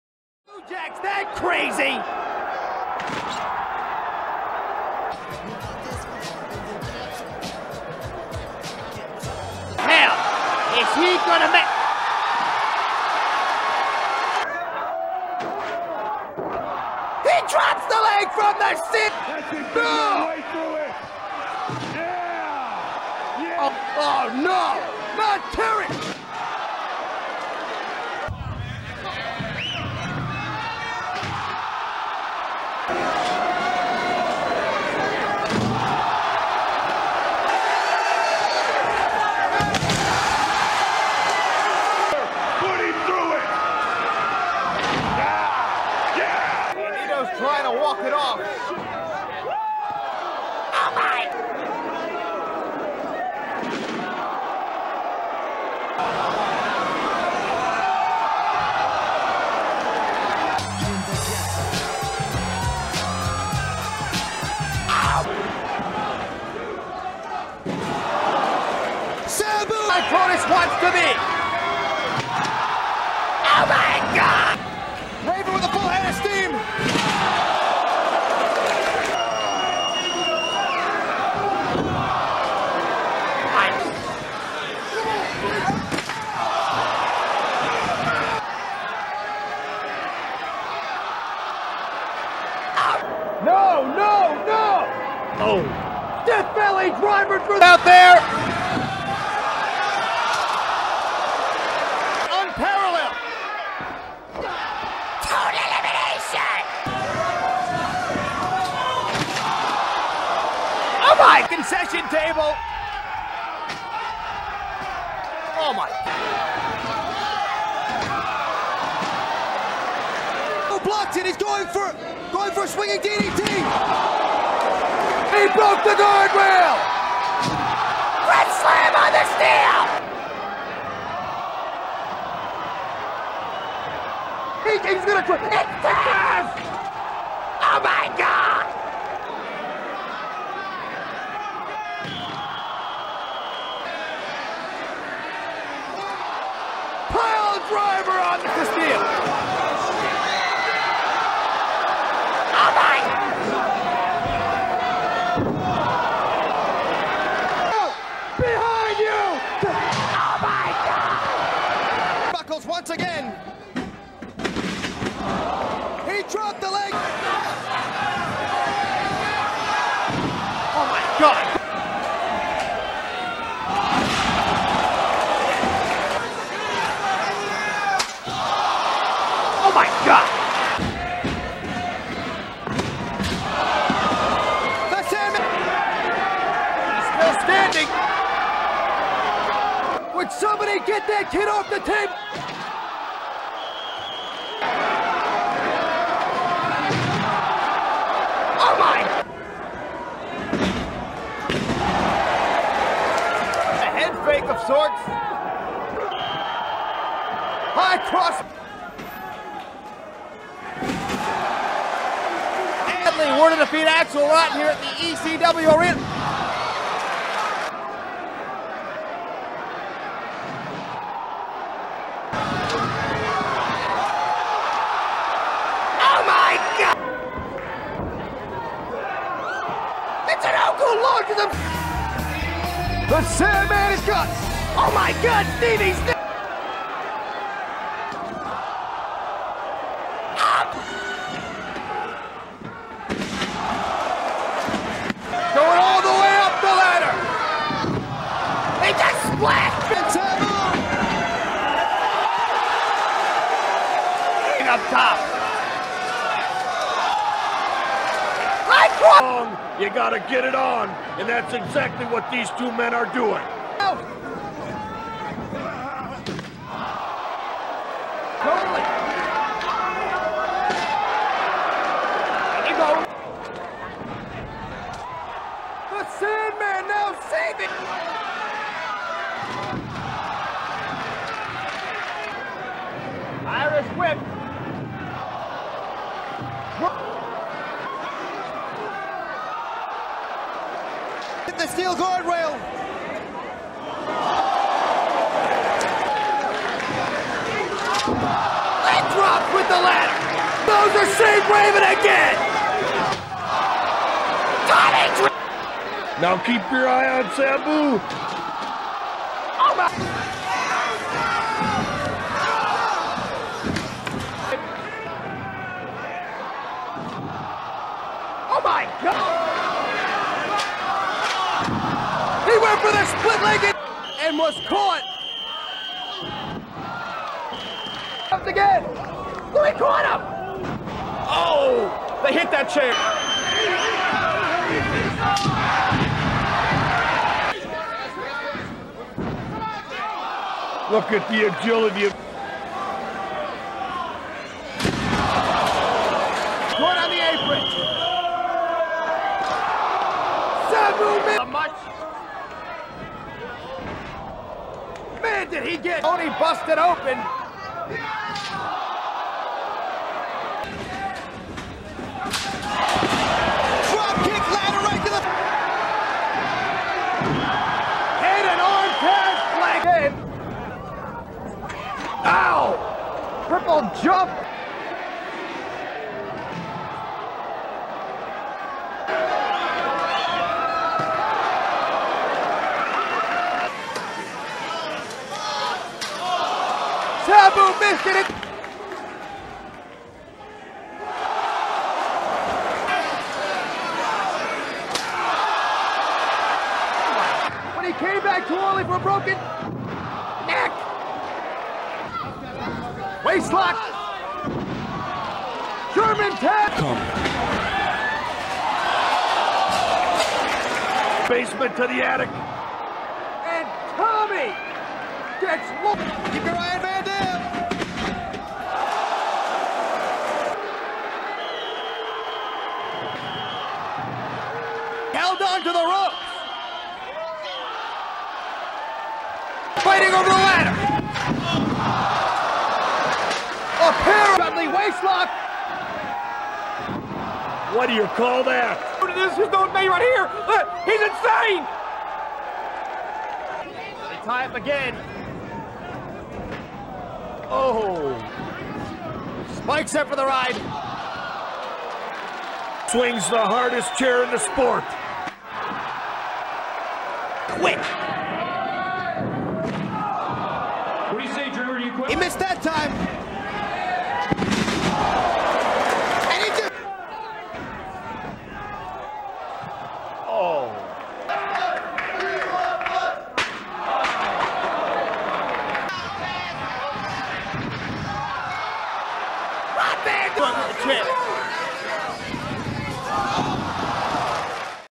ECW 100 OMG Moments 1997 Part 1
Hey guys and what's up MAH is back with another banger OMG Moments compilation video

We started the ECW OMG Moments journey with covering 1998  and now we're heading a year before
This is 1997 and we're highlighting some of the craziest moments in ECW that year

This ECW 100 OMG Moments of 1997 Part 1

This is a very stacked video and it's packed heavy with some violent and extreme moments  as 1997 was a  big year for the brand and it was the peak of the extreme promotion if we're being honest and it makes all sense with the talent and the feuds they had going it was hot

This video is crazy but I promise you part 2 is gonna be even more craziest  just wait and you'll see